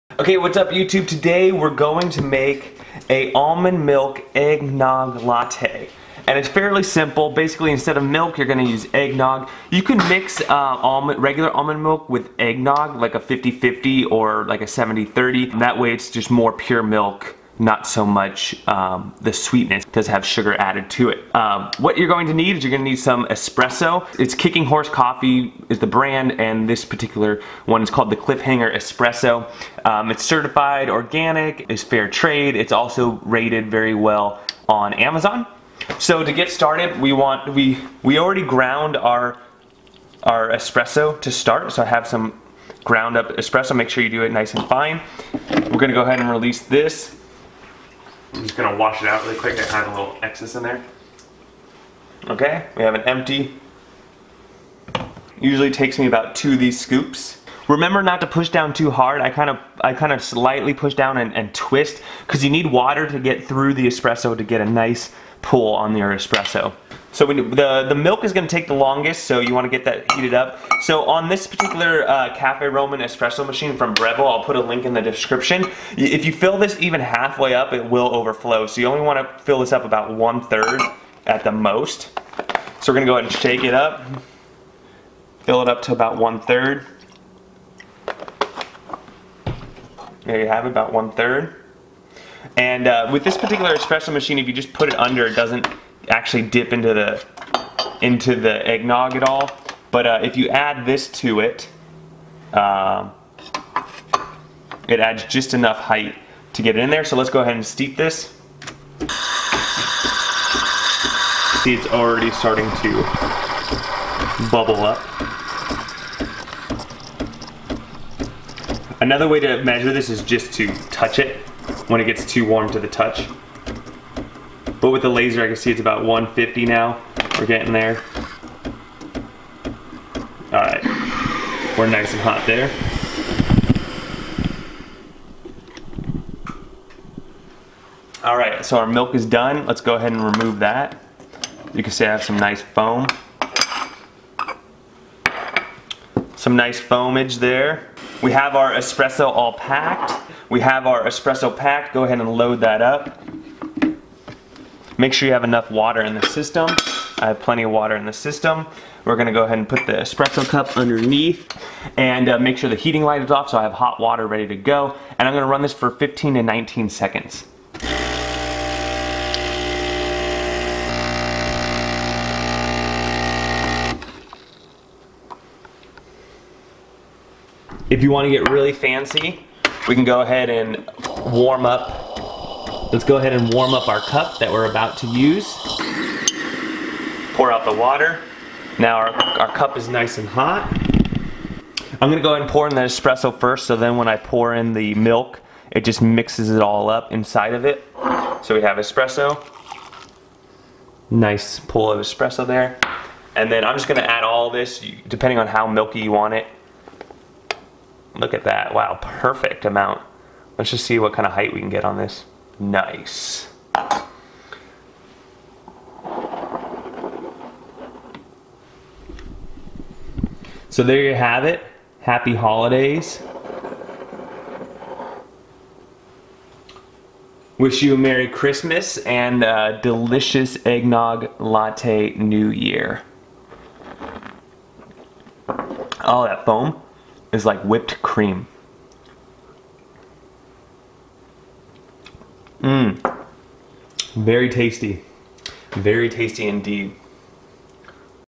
How To Make An Eggnog Latte
mmmmmmmmmm Eggnog Latte

This is the same process for regular eggnog or almond nog

Amazon link for espresso machine:

Amazon link for Crazy Horse Espresso (highly rated):